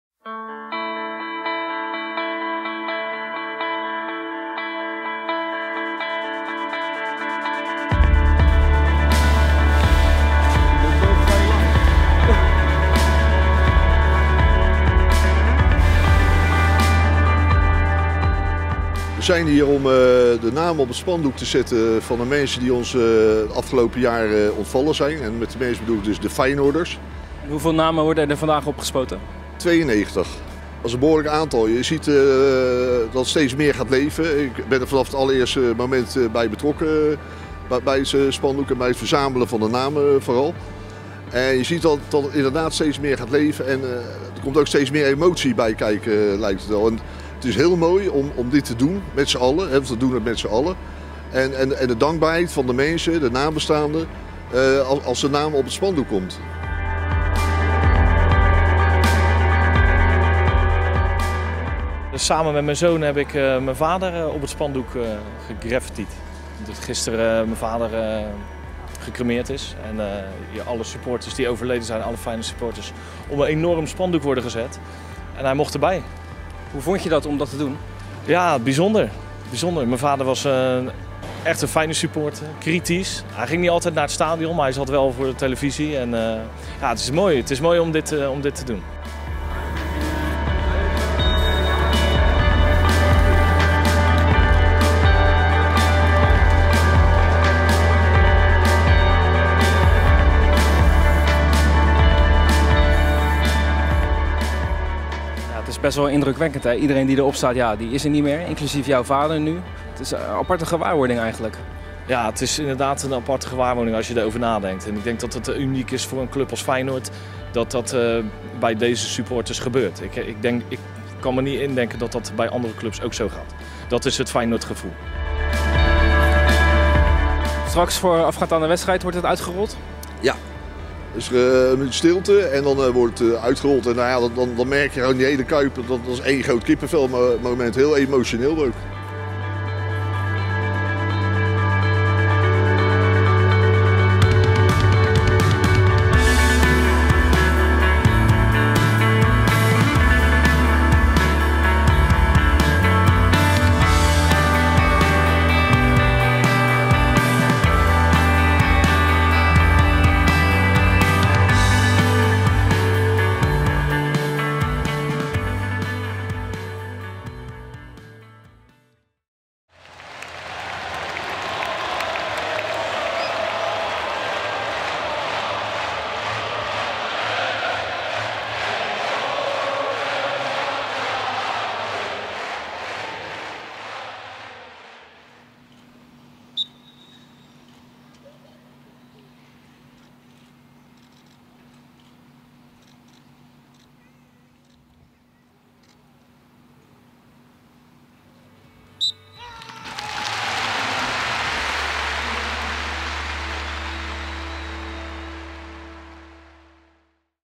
Het verhaal achter het herdenkingsdoek
Geheel volgens traditie werd bij de Openingswedstrijd van dit seizoen het herdenkingsdoek getoond in De Kuip. Hierop staan de namen van overleden Feyenoorders. Het doek zou er niet zijn zonder het werk van FSV De Feijenoorder. Bekijk de video voor de making of en het verhaal erachter.